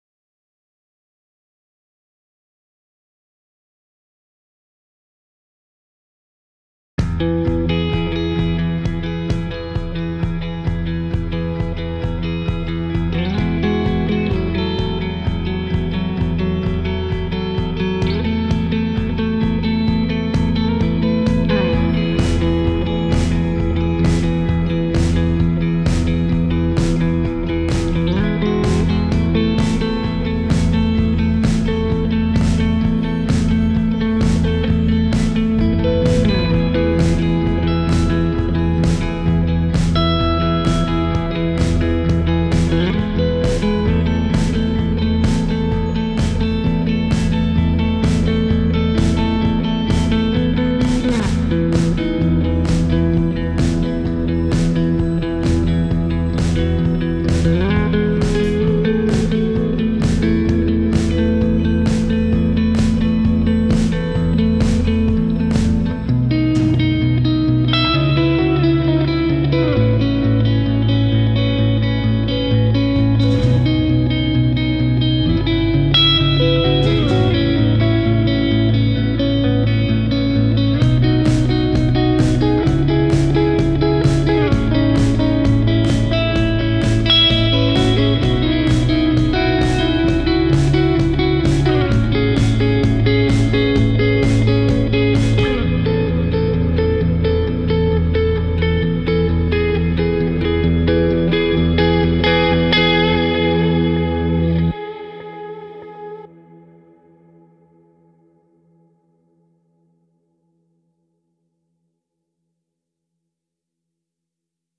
Jam Episode 1 / 2021
1am session with myself.

Mixed and Mastered thru Ableton Live suite
Fender Squire Telecaster HH
Focusrite Scarlett 2i2
Beyerdynamic Headphones 770 pro
ASM5 Swissonic Studio Monitors